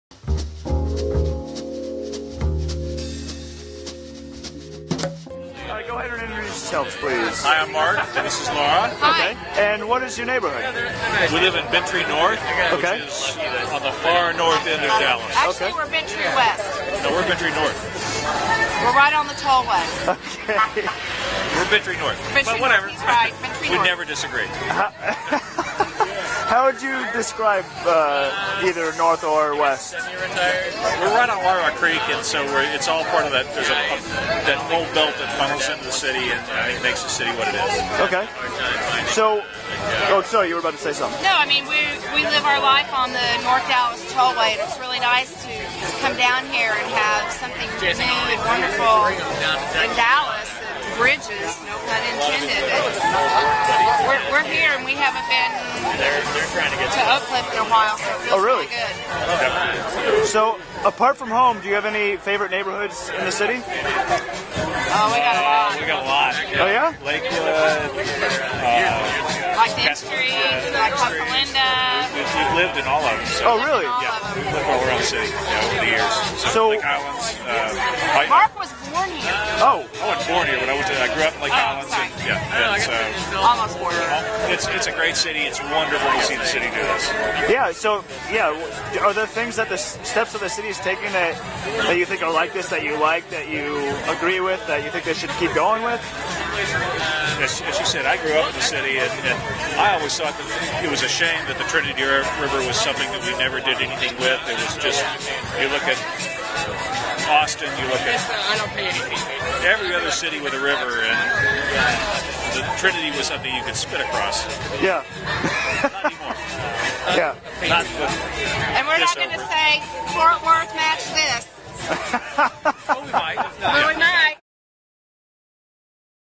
Laura Zeller & Mark Seeberger | Bent Tree North | Dallas
This interview was recorded on 3.3.2012 at the opening of the Margaret Hunt Hill Bridge in Dallas, Texas by buildingcommunityWORKSHOP as part of the Neighborhood Stories initiative.